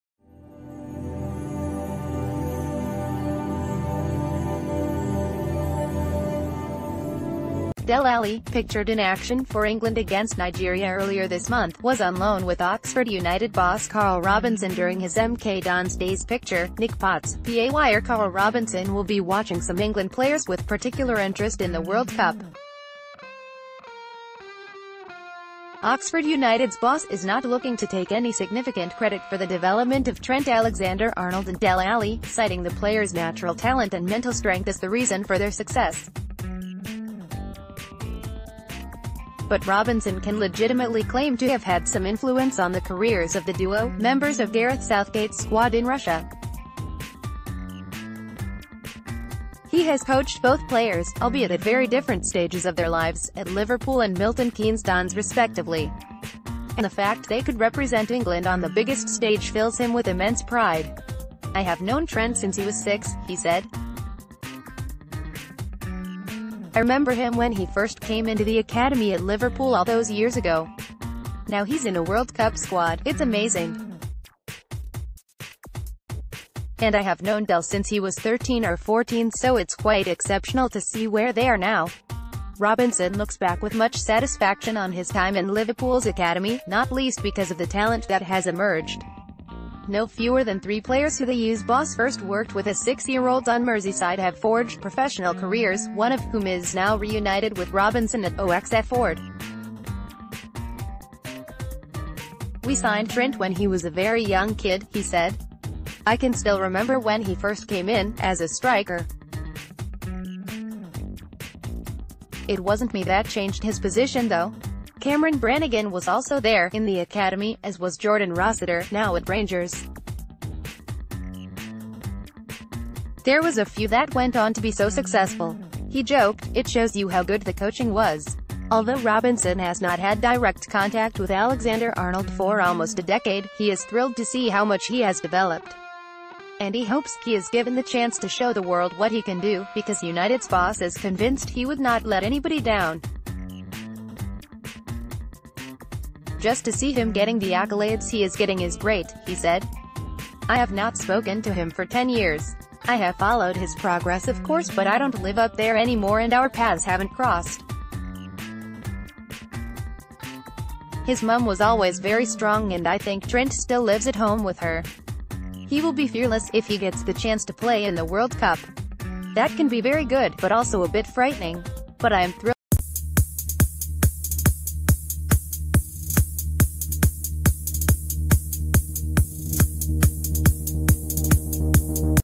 Breaking News | Oxford United boss Karl Robinson proud to see Dele Alli and Trent Alexander-Arnold
Breaking News | Oxford United boss Karl Robinson proud to see Dele Alli and Trent Alexander-Arnold bid for World Cup glory

Dele Alli, pictured in action for England against Nigeria earlier this month, was on loan with Oxford United boss Karl Robinson during his MK Dons days Picture: Nick Potts/PA Wire KARL Robinson will be watching some England players with particular interest in the World Cup. Oxford United’s boss is not looking to take any significant credit for the development of Trent Alexander-Arnold and Dele Alli, citing the players’ natural talent and mental strength as the reason for their success. But Robin...